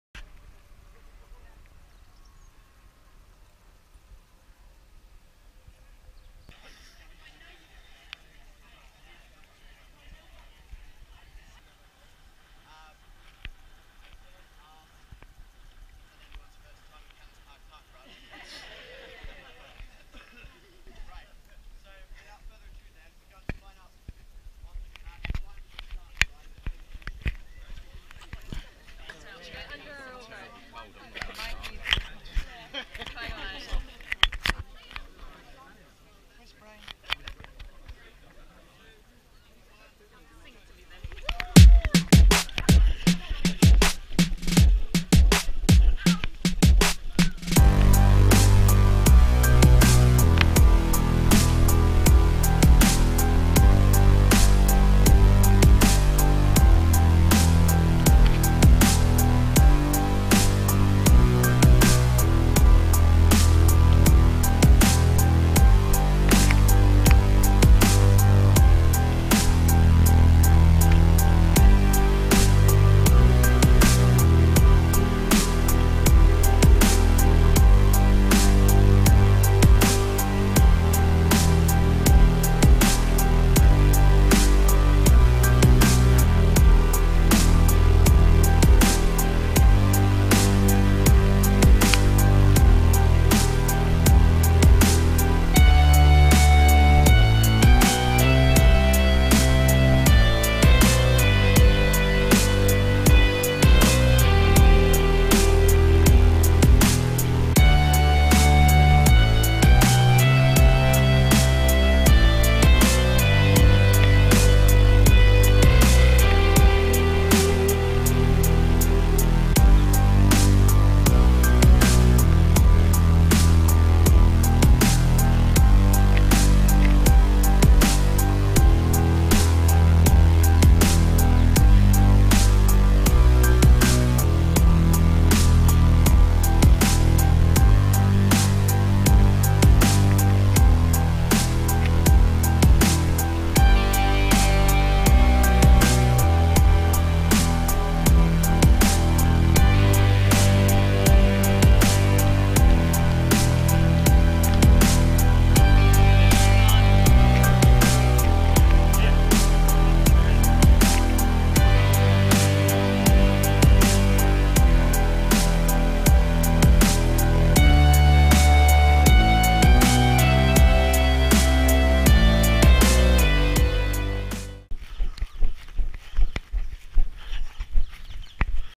Canons Park Inaugural Parkrun, 8th October 2016
The first lap and finish of the Canons Park course.